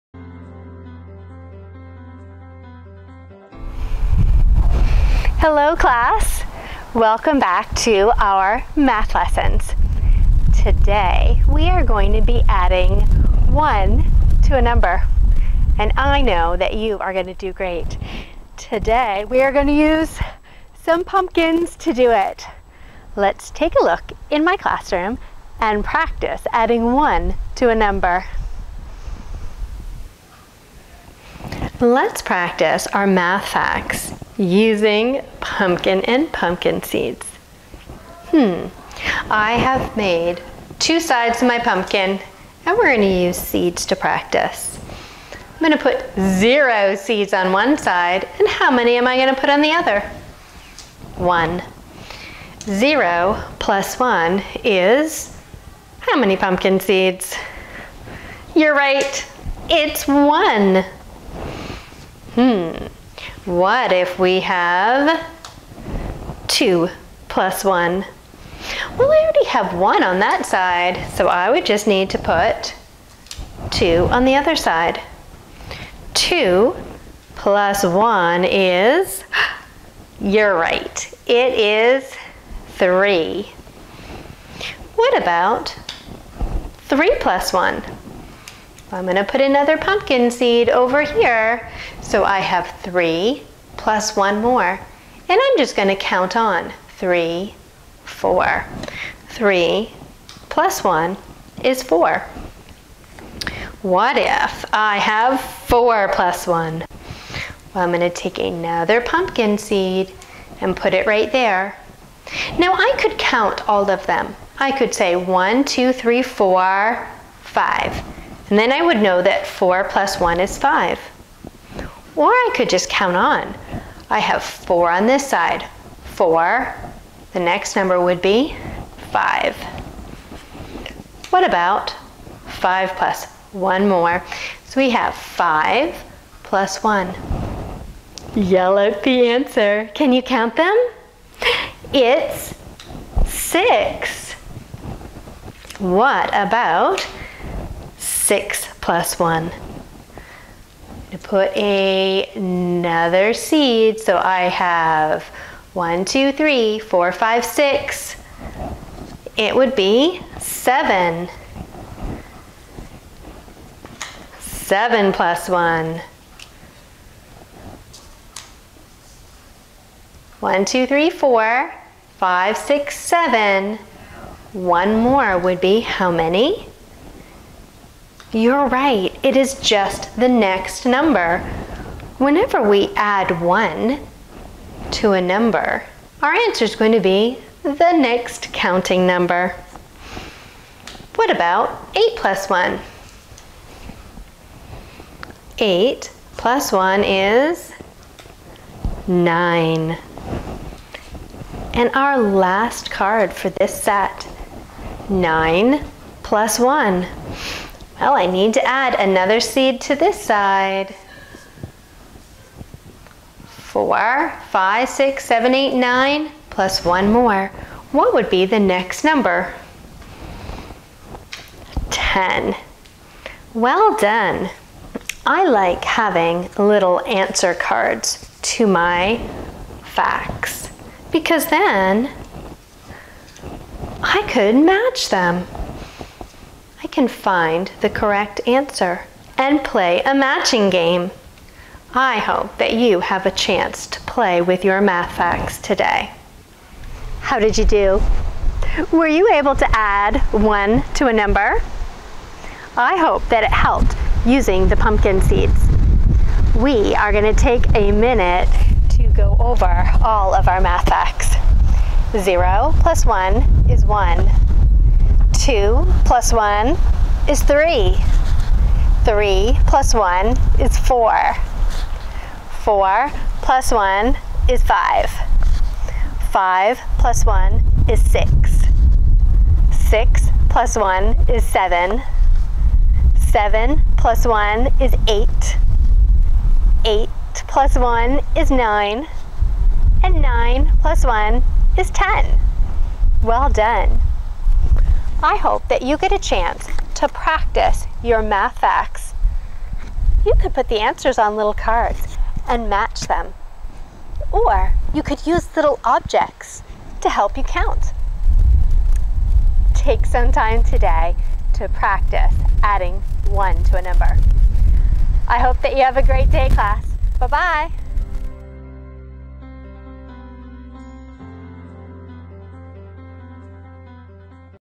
Grade 1: Math Lesson #37 Adding 1 To A Number
Welcome to our online first grade math class. Join me as we review adding 1 to a number.  Let's practice our plus one facts.

This video corresponds to the first grade (grade one) Saxon math curriculum lesson # Adding 1 To A Number.   It also introduces concepts included in the first grade math curriculum from Abeka, ACSI, Scott Foresman,  Harcourt Math, and Math You See.